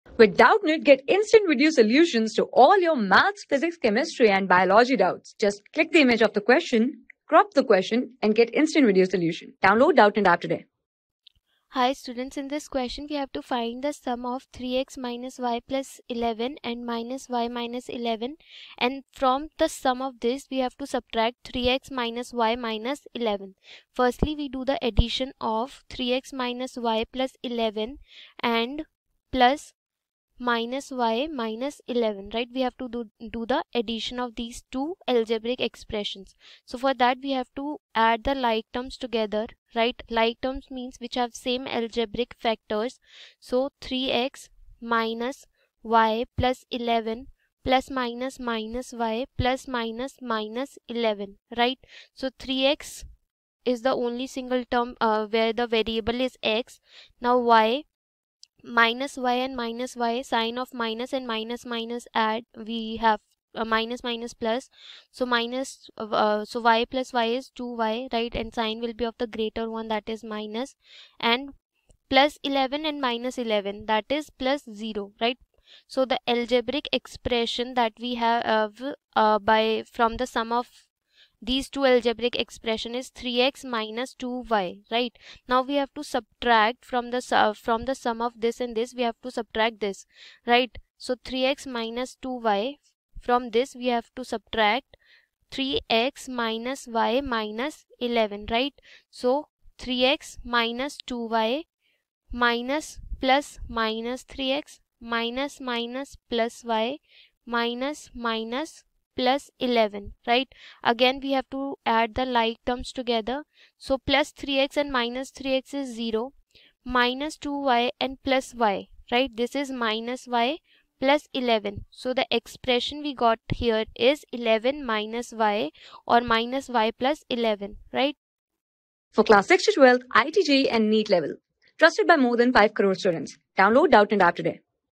From the sum of 3x-y+11 and -y-11 subtract 3x-y-11. | 7 | ALGEBRAIC EXPRESSIONS | MATHS | NCERT...
From the sum of 3x-y+11 and -y-11 subtract 3x-y-11.
Class: 7
Subject: MATHS
Chapter: ALGEBRAIC EXPRESSIONS
Board:CBSE

👉 Have Any Query? Ask Us.